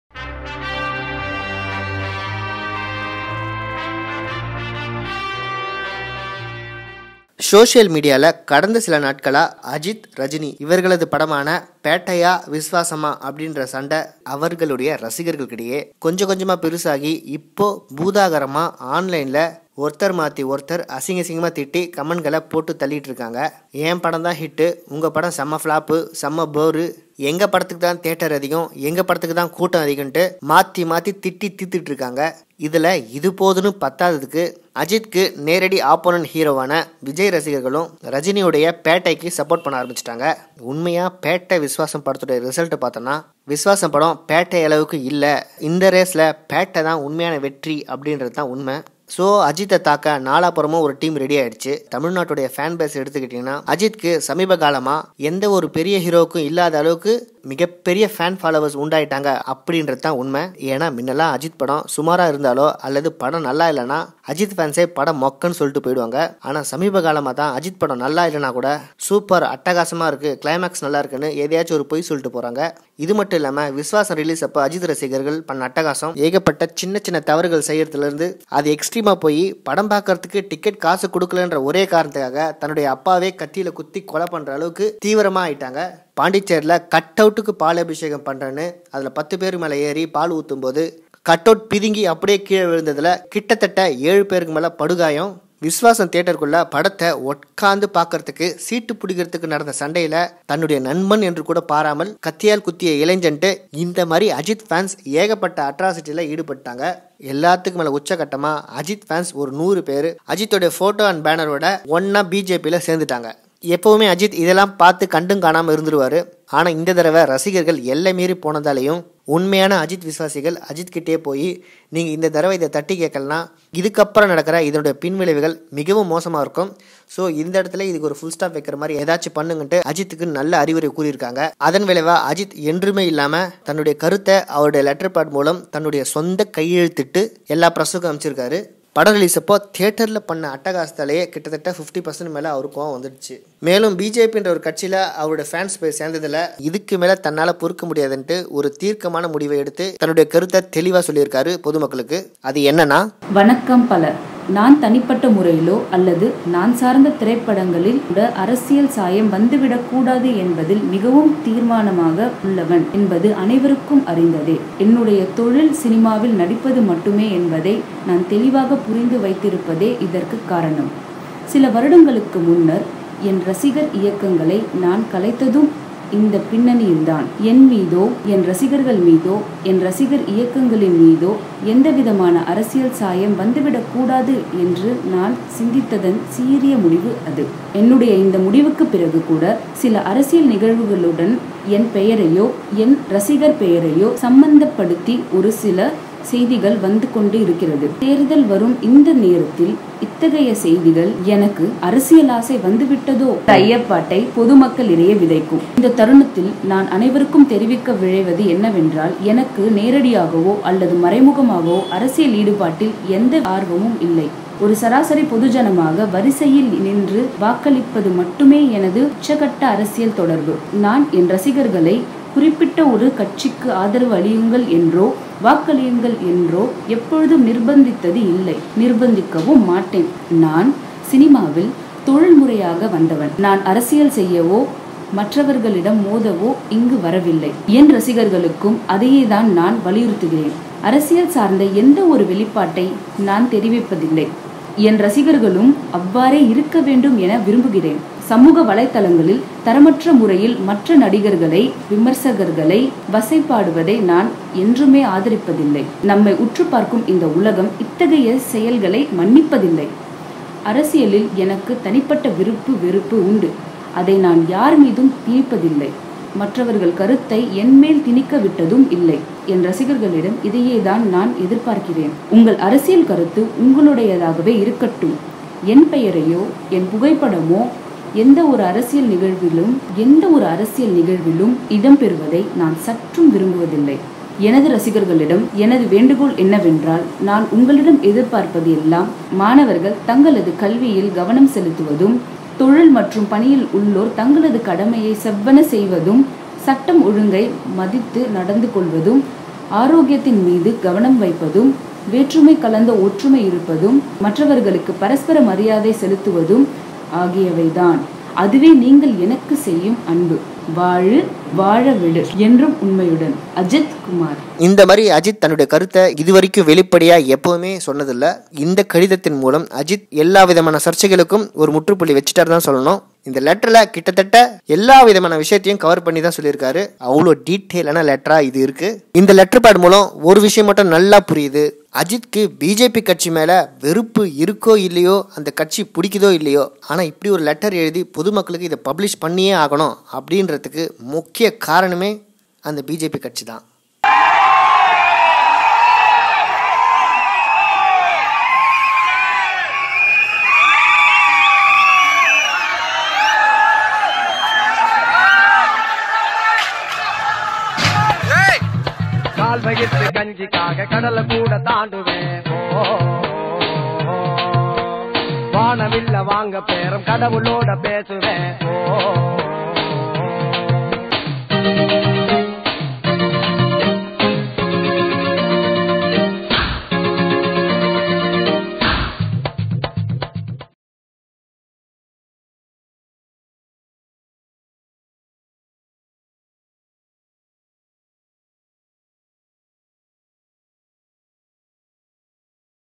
AJITH vs BJP க்கு பதிலடி தந்த தல | Thala Ajith Reply to Political Party & Fans | Ajith Press News
THALA FANS ATROCITY...
Viswasam vs Petta
Politics vs Ajith
Vaazhu Vaazha Vidu...
Ajith Politics Detailed Report...